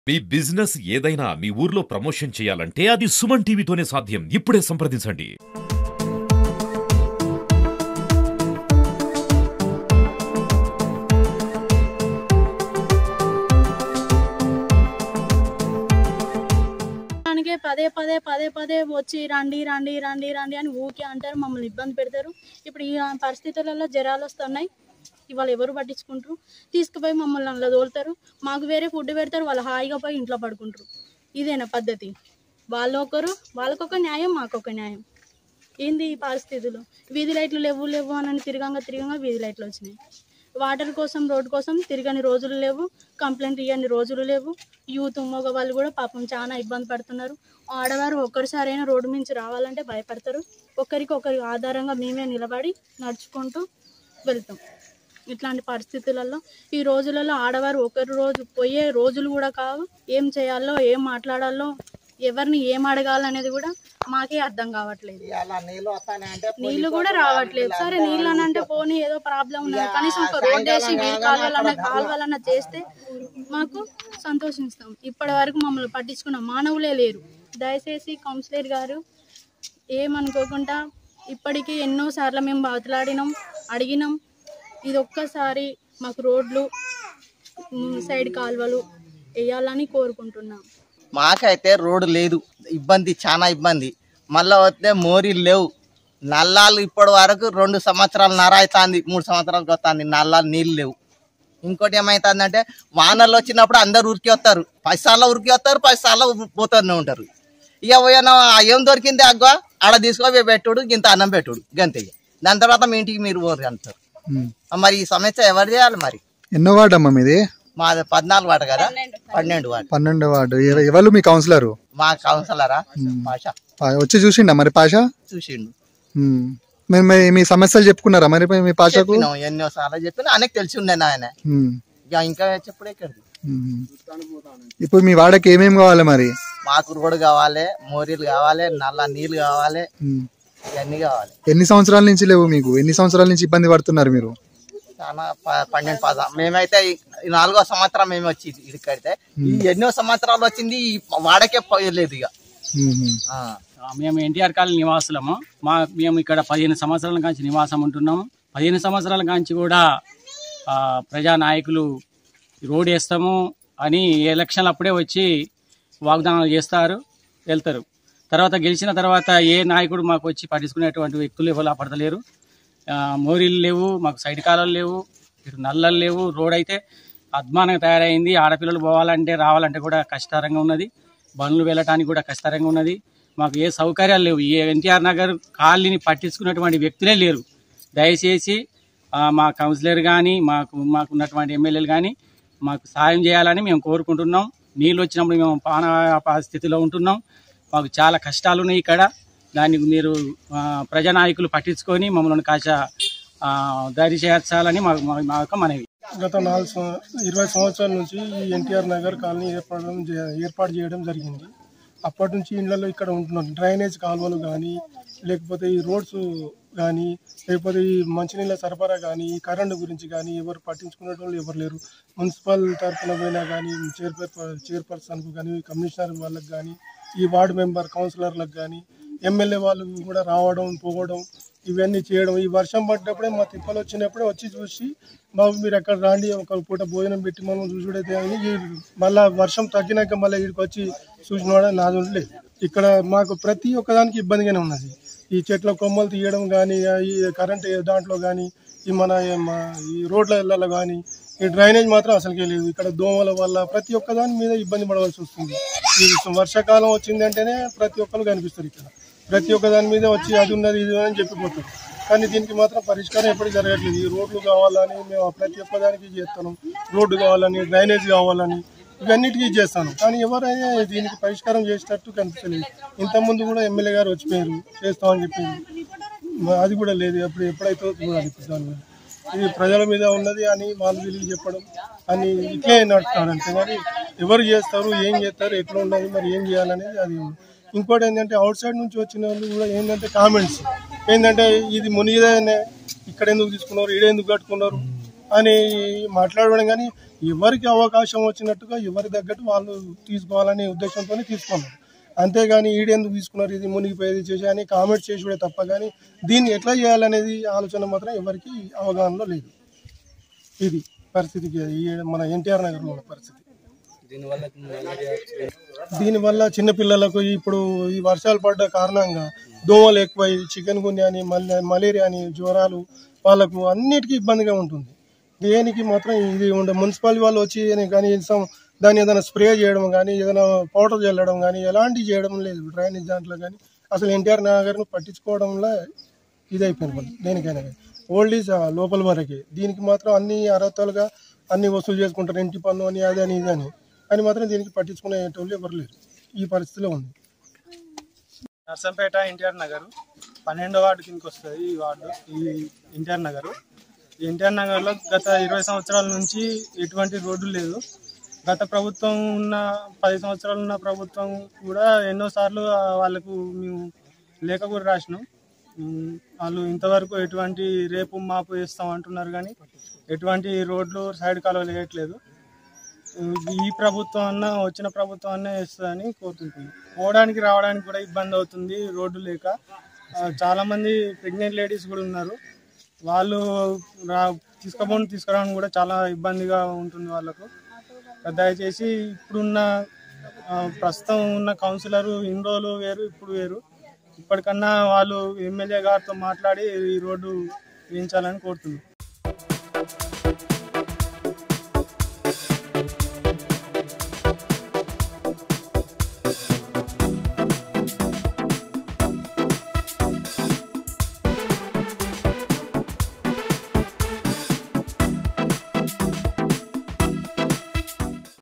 నర్సంపేట మున్సిపాలిటీ పరిధిలో సమస్యలు ఇబ్బందులు.. | Narsampet Municipality | NTR Nagar 12th Ward
Watch Here
 నర్సంపేట మున్సిపాలిటీ  పరిధిలో సమస్యలు ఇబ్బందులు | Narsampet Municipality | NTR Nagar 12th Ward | @sumantvwarangal981